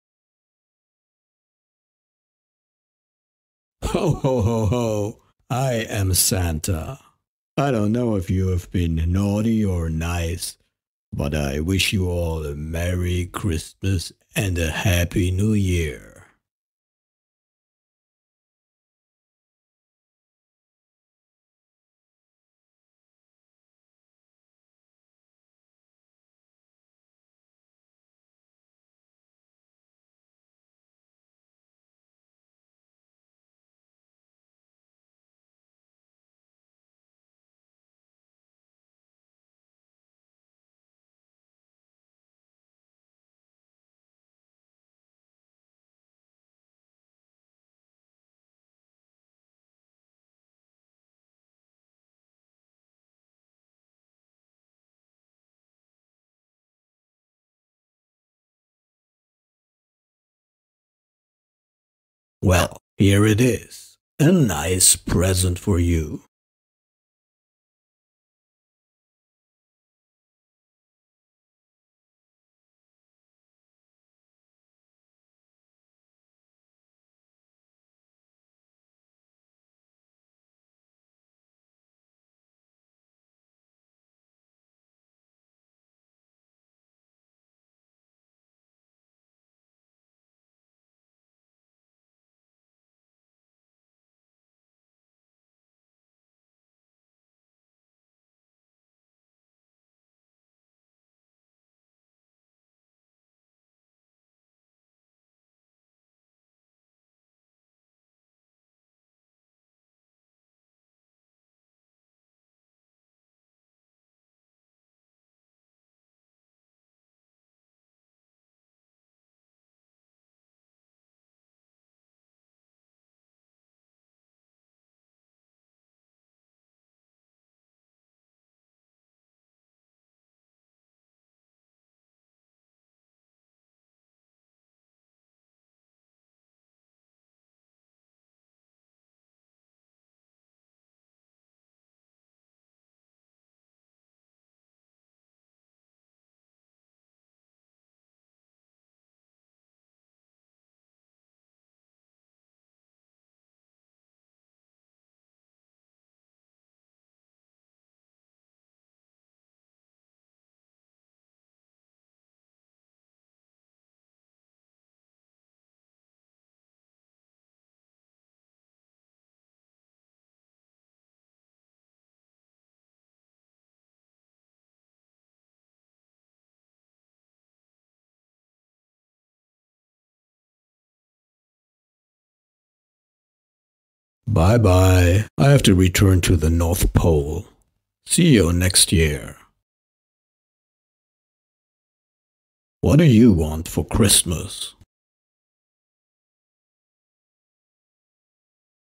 Santa Claus traditional hero update
new version demo video of my Santa Claus traditional hero character asset for Unity3d, now with improved PBR textures.